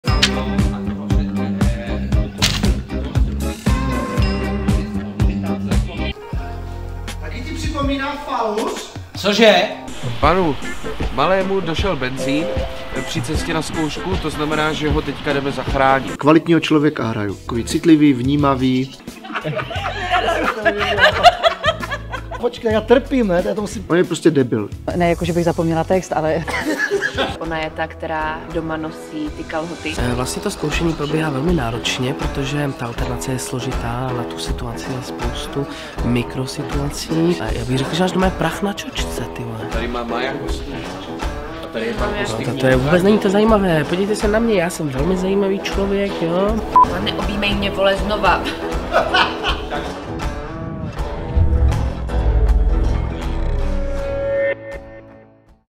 Making of Ledvina | sledujte na MirPlay.cz
💥 Making of Ledvina je na MírPlay! 🚀 Nejedná se o dobrodružný výlet za poznáním lidského těla, ale jedinečný videodokument o zákulisí vzniku naší napínavé inscenace! 🍲 Podívejte se pod pokličku, jak se taková hořká komedie vaří a následně se přijeďte od 30. září podívat do Třince na výsledek. 👇

🎭 Making of Ledvina vás teleportuje do období přelomu roku 2022 a 2023, kdy jsme hru intenzivně zkoušeli. Uvidíte celý proces vzniku od první čtené zkoušky až po premiéru. ❄️ Ve videu se kromě herců a inscenačního týmu mihne také sníh, což je v těchto vedrech vítané osvěžení. 📹 Těšit se můžete na rozhovory, zkoušky, bezprostřední a necenzurované reakce, ukázky z představení, a mnoho dalšího.

📺 MírPlay čeká, stačí kliknout na odkaz nebo otevřít svou aplikaci v mobilu a chytré TV. 📱